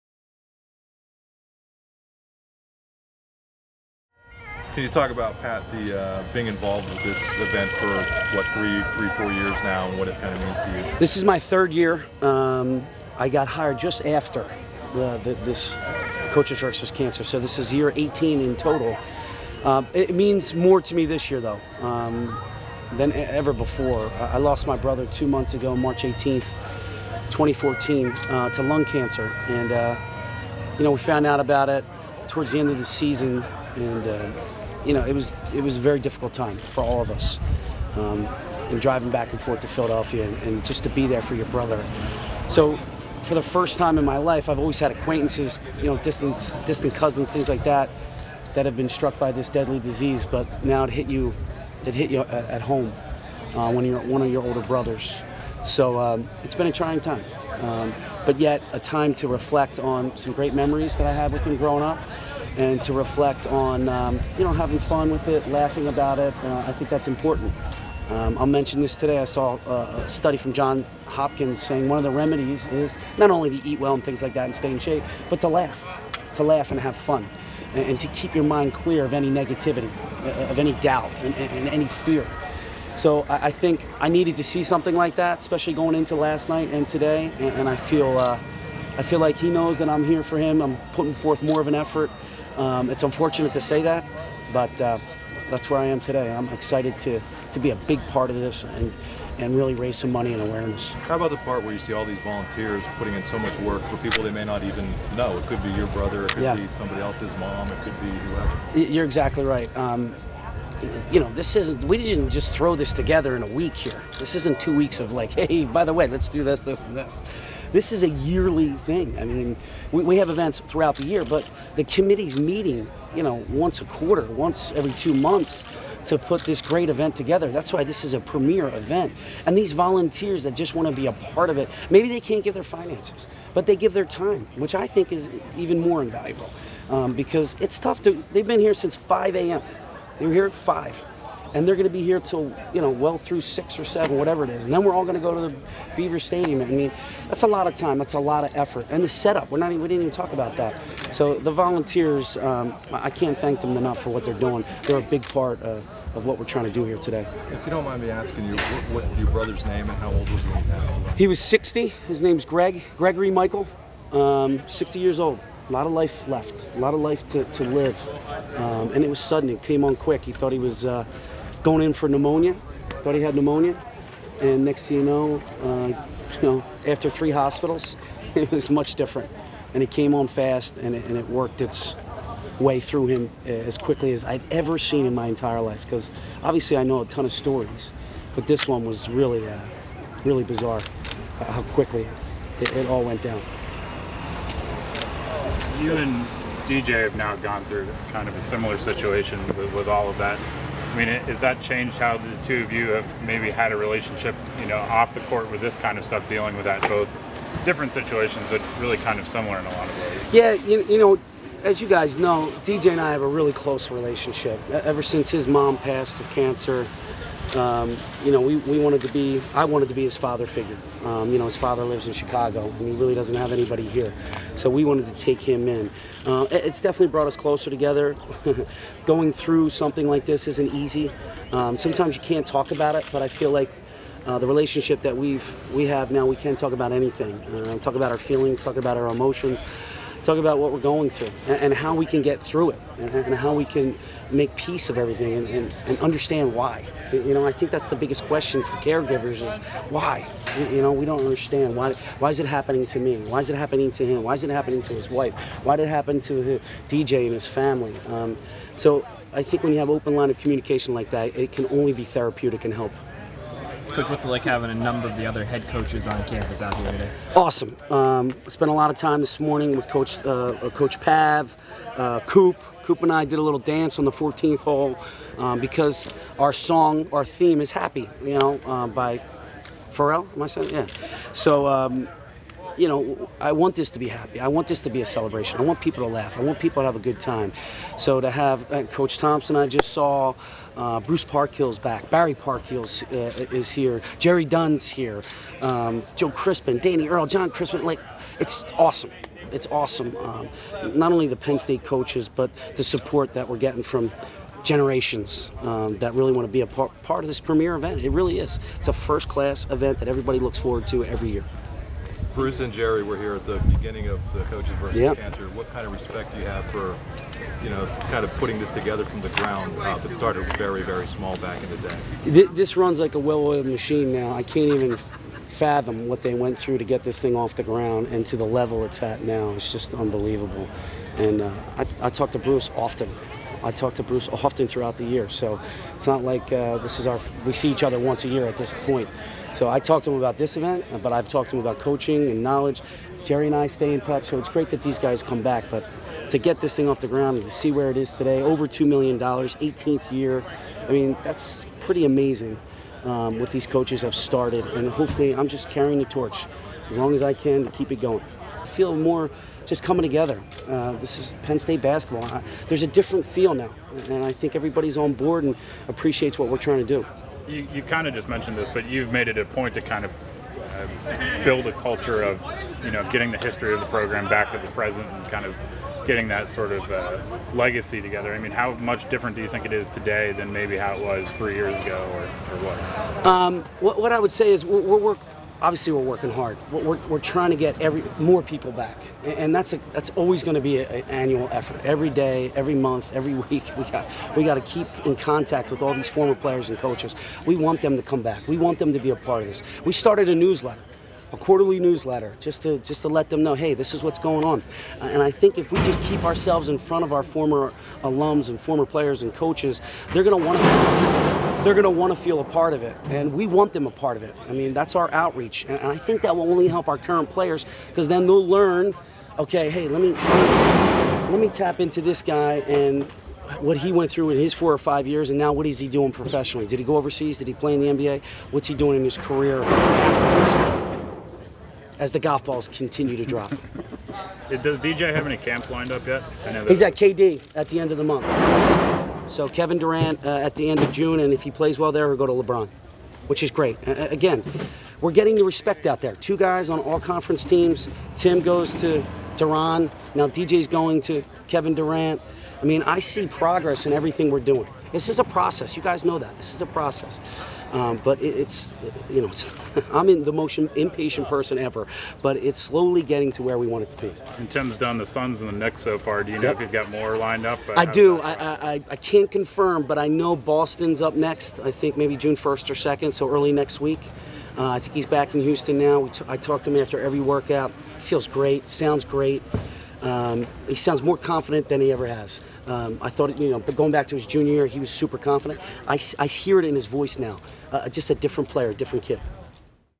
2014 Coaches vs. Cancer - Patrick Chambers Press Conference
Head coach Patrick Chambers meeting the media at the 2014 Coaches vs. Cancer Golf Outing.